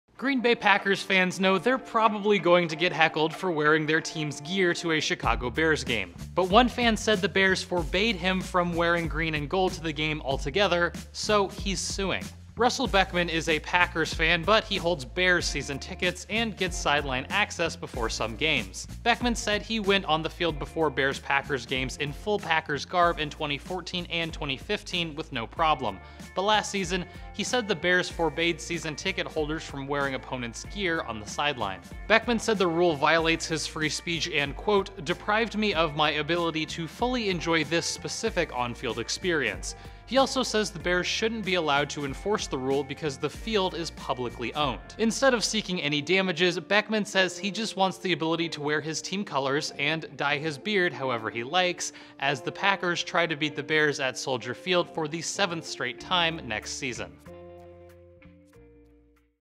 Packer fan sues to wear his jersey to Bears game
A Green Bay Packers fan filed a lawsuit after he said he was barred from wearing his team's colors to Chicago Bears events.